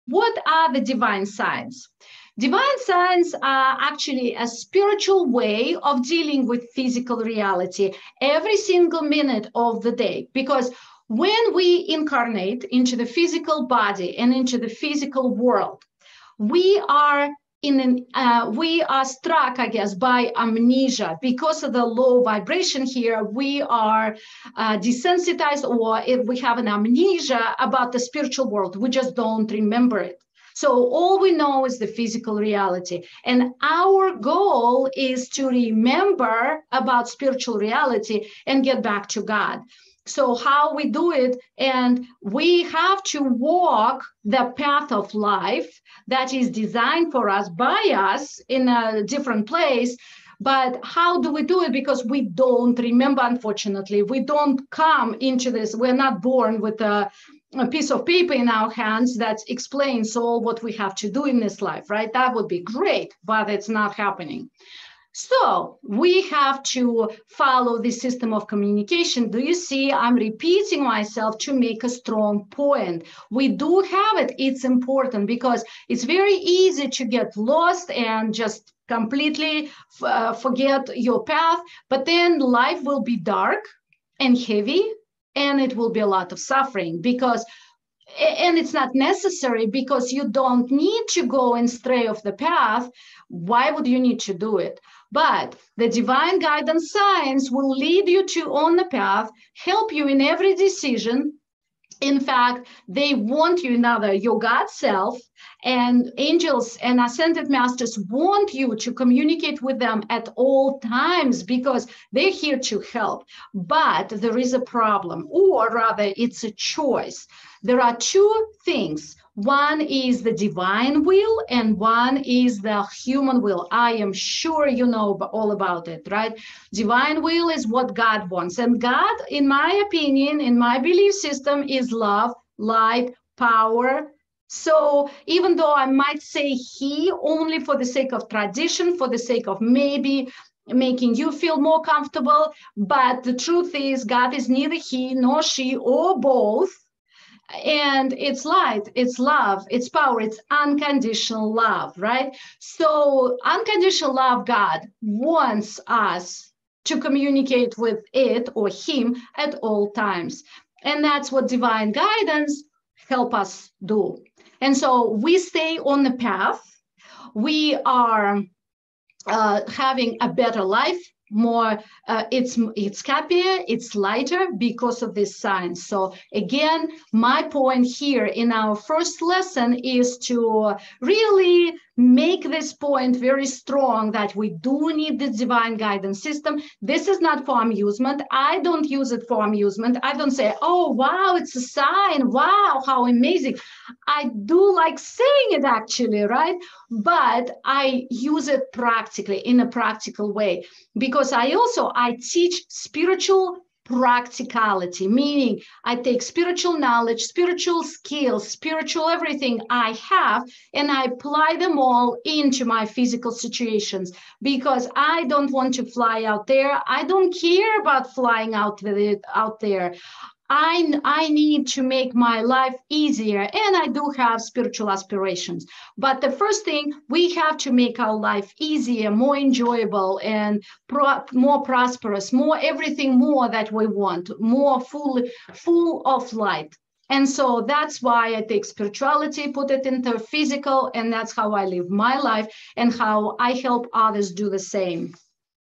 Free vs Divine Will In the Divine Guidance Signs System
Why not many use the Divine Guidance Signs in daily life. Free Will is not to be interfered with.
This video is an excerpt from Irina's teaching.